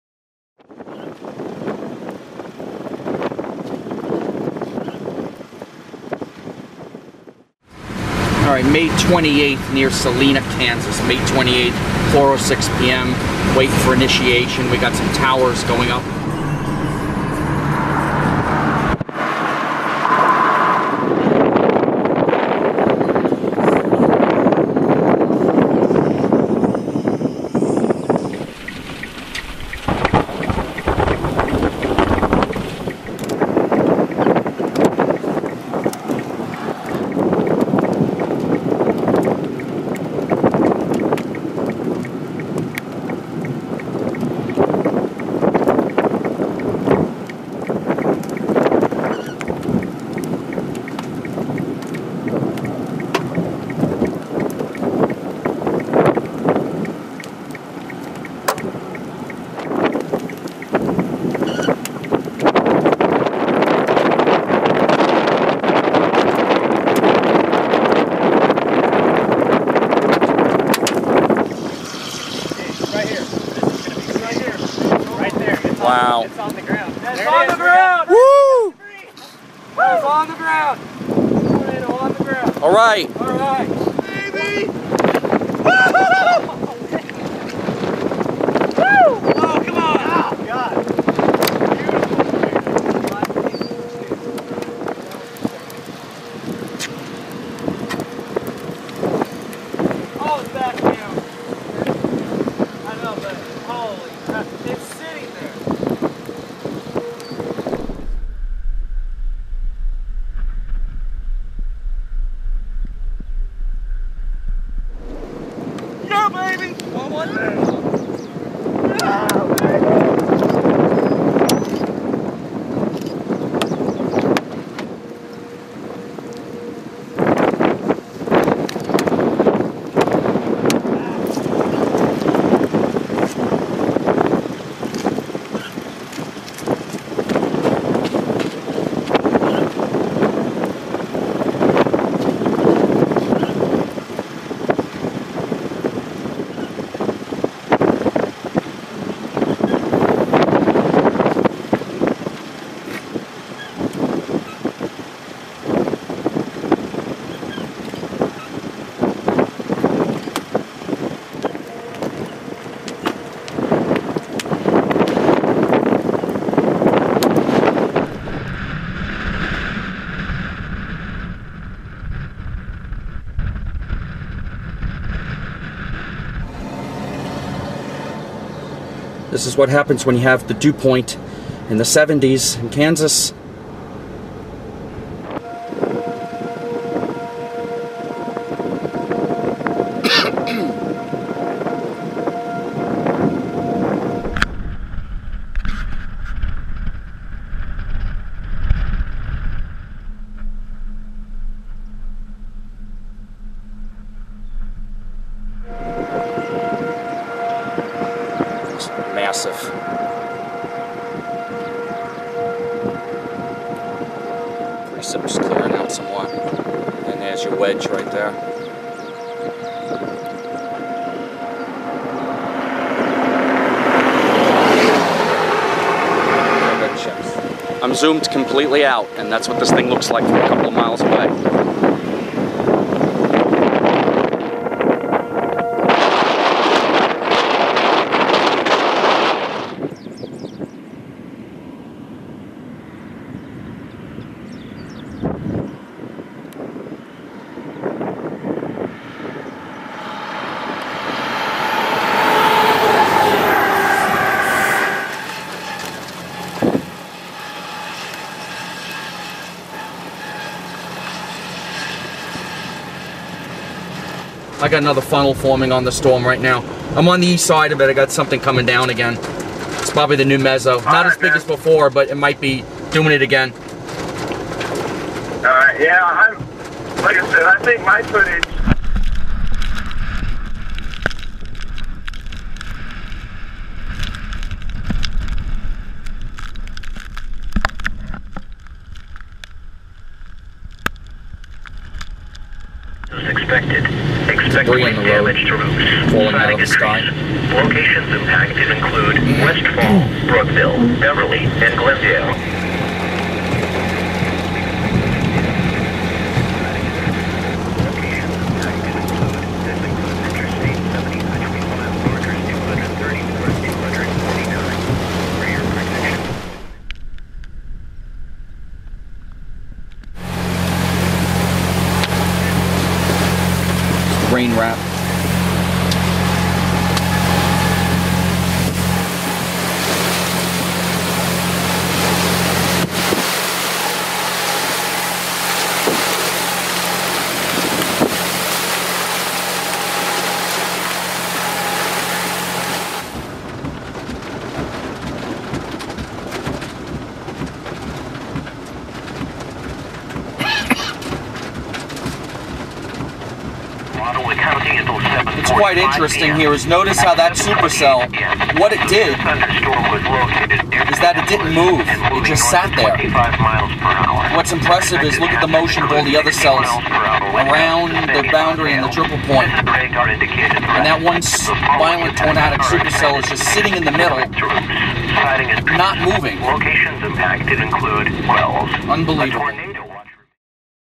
Violent EF-4 Tornado Near Bennington KS On May 28, 2013
This is a video of the violent (EF-4) tornado in Ottawa County, Kansas near Bennington during the late afternoon of May 28, 2013. This was an impressive supercell storm developing along the intersection of a stationary frontal boundary and dryline to the northwest of Salina, Kansas and west of I-135. This tornado was 1.4 MILES wide and was on the ground for OVER an hour, packing winds close to 200 MPH! See the beautiful life cycle of this severe weather event, in addition to the storm remaining over open farmland and not hurting anyone.